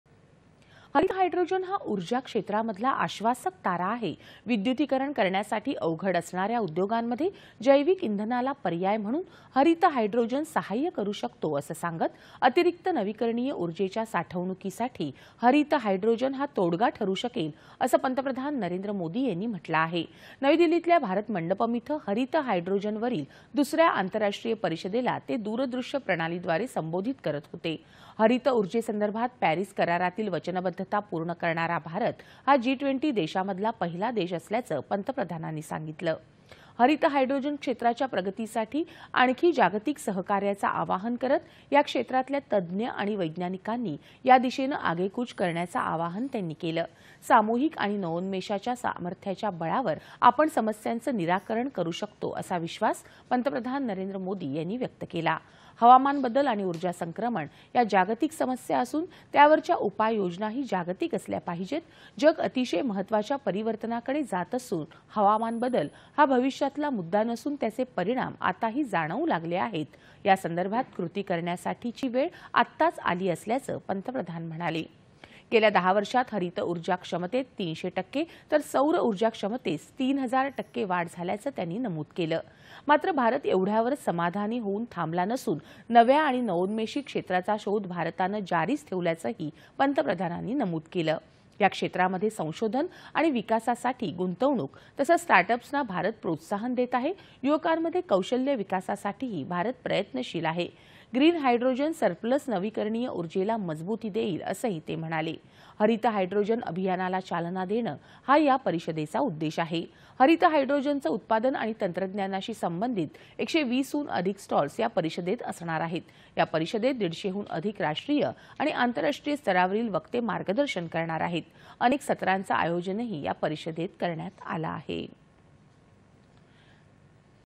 पंतप्रधान यांनी हरीत हायड्रोजनवरील दुसऱ्या आंतरराष्ट्रीय परिषदेला ते दूरदृश्य प्रणालीद्वारे संबोधित
दि.११.०९.२०२४ :
हरित हायड्रोजन हा उर्जा क्षेत्रामधला आश्वासक तारा आहे.  विद्युतीकरण करण्यासाठी अवघड असणाऱ्या उद्योगांमध्ये जैविक इंधनाला पर्याय म्हणून हरित हायड्रोजन सहाय्य करु शकतो असं  सांगून, अतिरिक्त नवीकरणीय उर्जेच्या साठवणूकीसाठी, हरित हायड्रोजन हा तोडगा  ठरु शकेल असं पंतप्रधान नरेंद्र मोदी यांनी म्हटलं आहे.   नवी दिल्लीतल्या भारत मंडपम इथं,  हरीत हायड्रोजनवरील दुसऱ्या आंतरराष्ट्रीय परिषदेला ते दूरदृश्य प्रणालीद्वारे   संबोधित करत होते.   हरित उर्जेसंदर्भात पॅरीस करारातली वचनबद्धता पूर्ण करणारा भारत, हा जी-20 देशामधला पहिला देश असल्याचंही पंतप्रधान म्हणाले.
हरित हायड्रोजन क्षेत्राच्या प्रगतीसाठी आणखी जागतिक सहकार्याचं आवाहन करत या क्षेत्रातल्या तज्ज्ञ आणि वैज्ञानिकांनी या दिशेनं आगेकूच करण्याचं आवाहन त्यांनी केलं.  सामुहिक आणि नवोन्मेषाच्या सामर्थ्याच्या बळावर आपण समस्यांचं निराकरण करु शकतो असा विश्वास, पंतप्रधान नरेंद्र मोदी यांनी व्यक्त केला आहे.
 हवामान बदल आणि उर्जा संक्रमण या जागतिक समस्या असून, त्यावरच्या उपाययोजनाही जागतिक असल्या पाहिजे. जग अतिशय महत्वाच्या परिवर्तनाकडे जात असून हवामान बदल हा भविष्यातला मुद्दा नसून त्याचे परिणाम आताही जाणवू लागले आहेत, यासंदर्भात कृती करण्यासाठीची वेळ आत्ताच आली असल्याचंही पंतप्रधान म्हणाले.
गेल्या १० वर्षात हरित उर्जा क्षमतेत ३०० टक्के तर सौर उर्जा क्षमतेत ३ हजार टक्के वाढ झाल्याचं त्यांनी सांगितलं.  मात्र भारत एवढयावरच समाधानी  होऊन थांबला नसून, नव्या आणि नवोन्मेषी क्षेत्राचा शोध भारतानं जारीच ठेवल्याचंही पंतप्रधानांनी नमूद केलं.  या क्षेत्रामध्ये संशोधन आणि विकासासाठी गुंतवणूक तसंच स्टार्टअप्सना भारत प्रोत्साहन देत आहे.  युवकांमध्ये कौशल्य विकासासाठीही भारत प्रयत्नशील आहे.  ग्रीन हायड्रोजन सरप्लस नवीकरणीय उर्जेला मजबूती देईल, असंही ते म्हणाले.
हरित हायड्रोजन अभियानाला चालना देणं हा या परिषदेचा उद्देश आहे.  हरित हायड्रोजनचं उत्पादन आणि तंत्रज्ञानाशी संबंधीत १२० हून अधिक स्टॉल्स या परिषदेत असणार आहेत.  या परिषदेत दिडशेहून अधिक राष्ट्रीय आणि आंतरराष्ट्रीय स्तरावरील वक्ते मार्गदर्शन करणार आहेत.  अनेक सत्रांचं आयोजनही या परिषदेत करण्यात आलं आहे.

8.30am | 11.00am | 1.00pm | 4.30pm | 7.00pm | 9.30pm